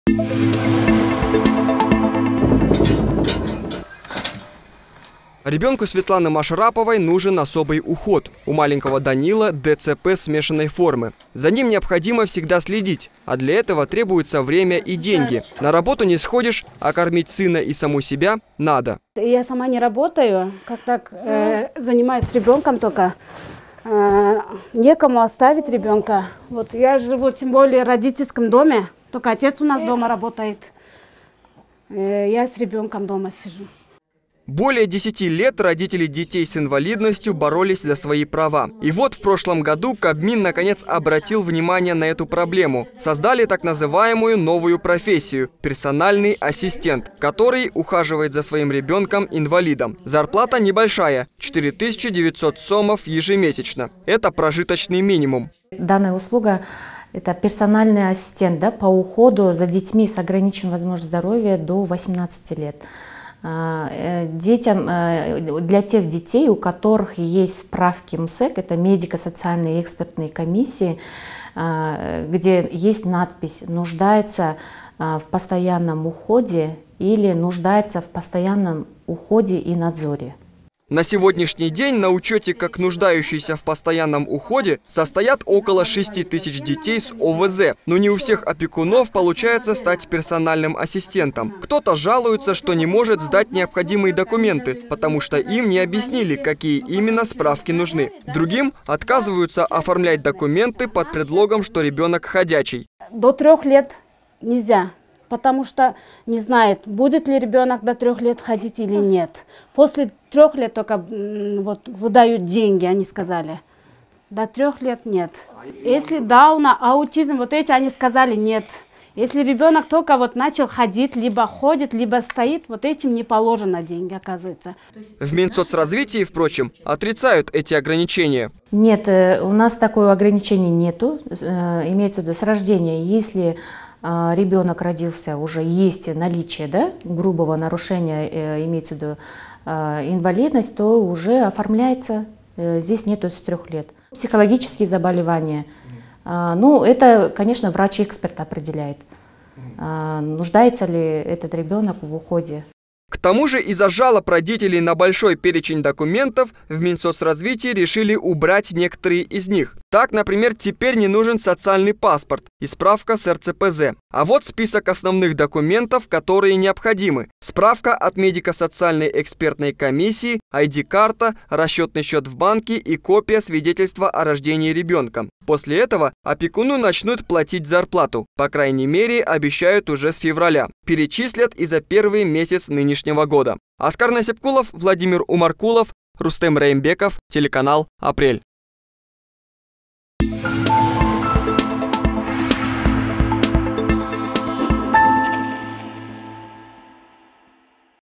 Список документов для оформления пособия по уходу за ребенком-инвалидом \\ Апрель ТВ
"Чиновники вновь нас обманули", - так жалуются родители детей с ограниченными возможностями здоровья, которым обещали уже с этого года выплачивать зарплату за уход за детьми с инвалидностью.
По словам родителей, чтобы оформить так называемую услугу "Персональный ассистент", чиновники требуют кучу различных справок. А когда их им приносят, и вовсе отказывают в приеме документов.